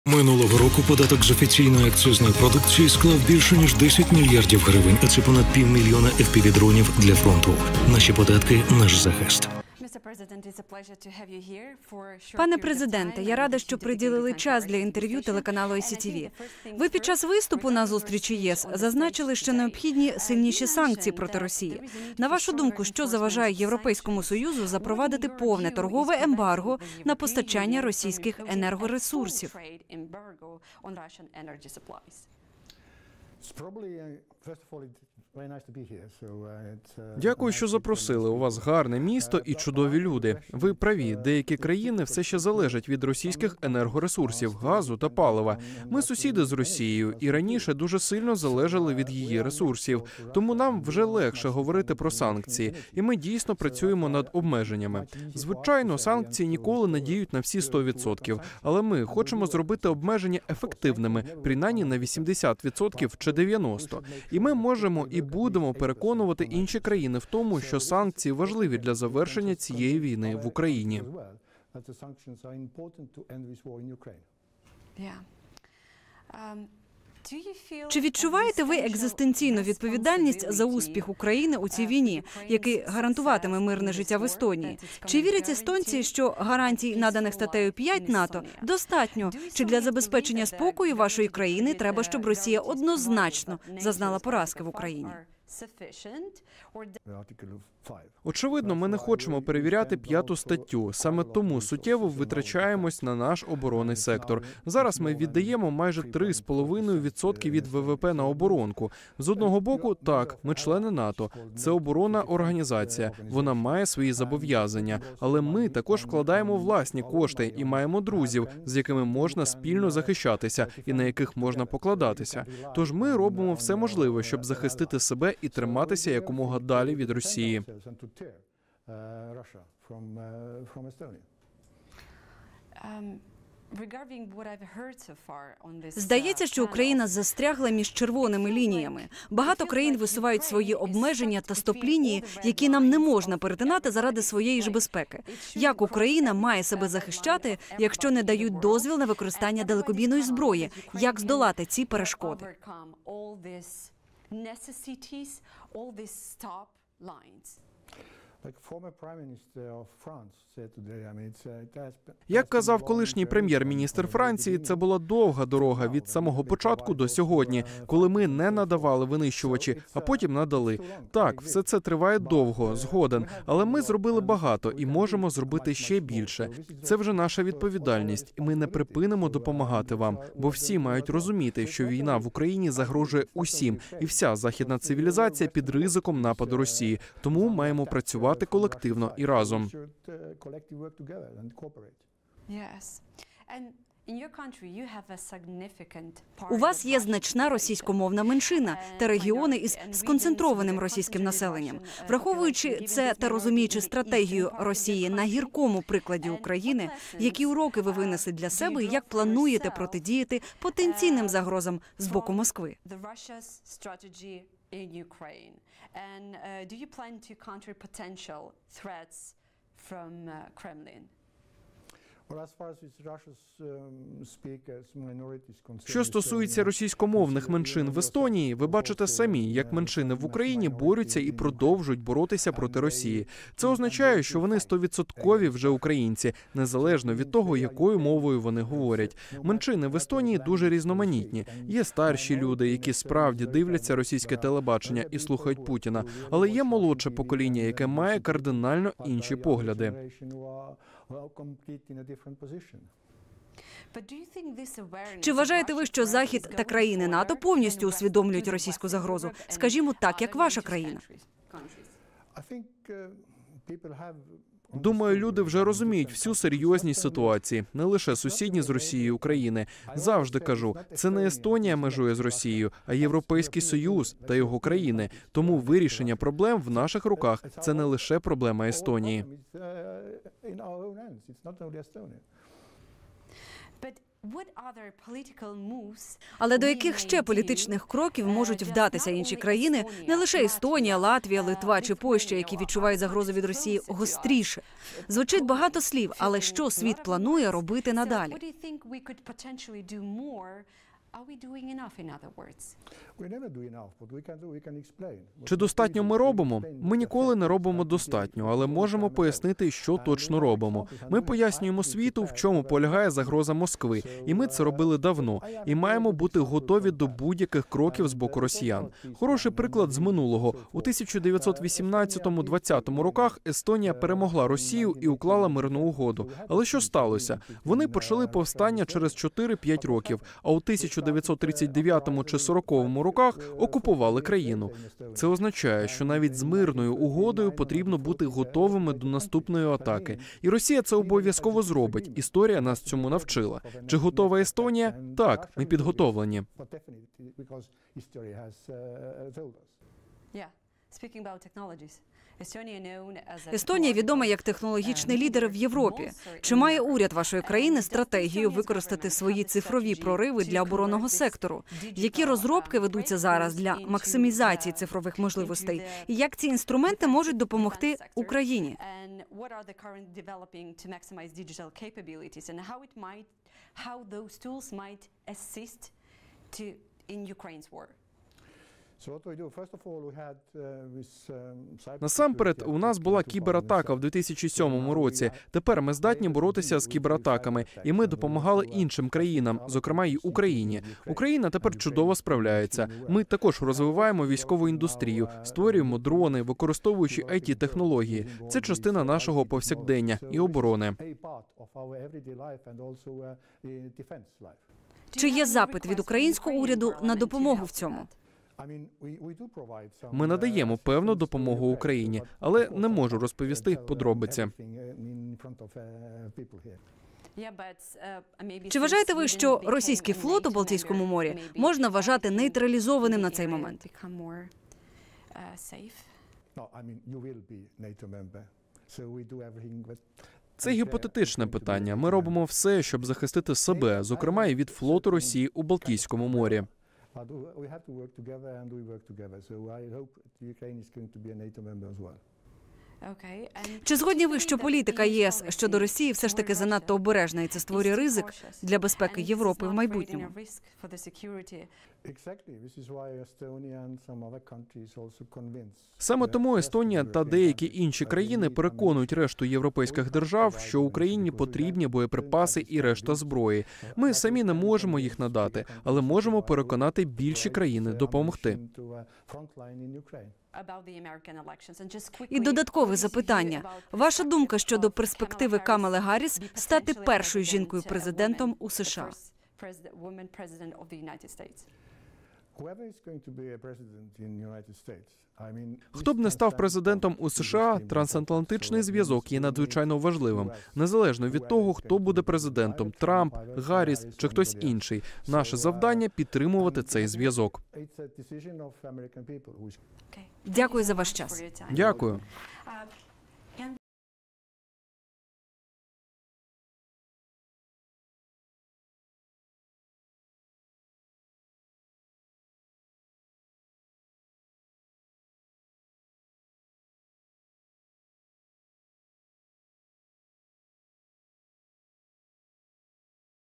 Ось у чому полягає ЗАГРОЗА ВІД МОСКВИ 🤯 ШОКУЮЧЕ інтерв'ю з ПРЕЗИДЕНТОМ ЕСТОНІЇ
Президент Естонії Алар Каріс дав інтерв'ю спеціальній кореспондентці ICTV Наталії Луценко, в якому розповів про позицію своєї країни стосовно війни та як вона може допомогти Україні.

00:10 – Початок
00:15 – Що заважає ЄС запровадити повне ембарго на російську енергетику
01:15 – Чи відчуває Естонія відповідальність за перемогу України
02:25 – Як Україна має себе захищати
03:35 – Як Естонія планує готуватися до агресії Росії
00:04:42 – Що світ планує робити далі
06:35 – Чи буде Естонія використовувати свою техніку на війні Росії проти України
07:00 – Чи російський флот нейтралізовано
07:20 – Чи вважає Алар Каріс, що політика ЄС щодо України занадто стримана
07:50 – Думки стосовно перспективи Гарріс стати президенткою США
08:20 – Кінець